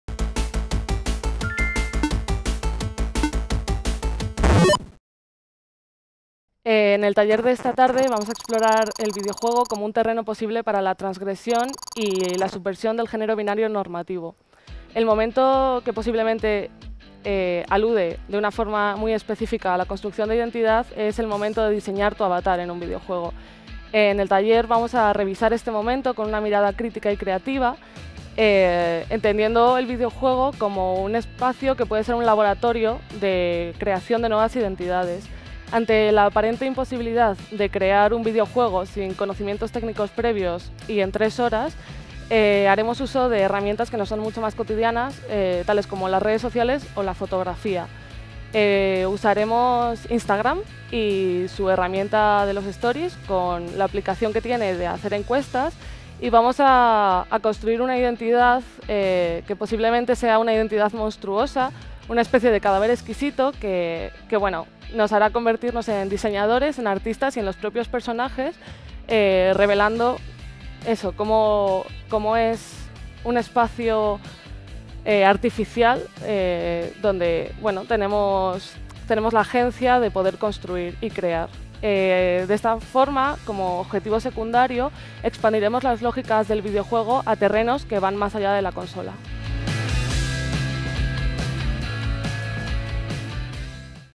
PDI. Videojuegos y mujeres: Clara Harguindey (Final Round)
En el taller 'Videojuegos e identidad: diseñando personajes en el ciberespacio', Clara Harguindey propone explorar el videojuego como un terreno posible para la transgresión y subversión del género binario normativo. Como objetivo secundario, el interés está en conocer el lenguaje del videojuego y experimentar con las lógicas interactivas de la construcción de relatos, entendiendo el juego como un medio posible para el conocimiento y la comunicación que va más allá de las consolas.

Un nuevo espacio para una nueva cultura: visita el Espacio Fundación Telefónica en pleno corazón de Madrid, en la calle Fuencarral 3.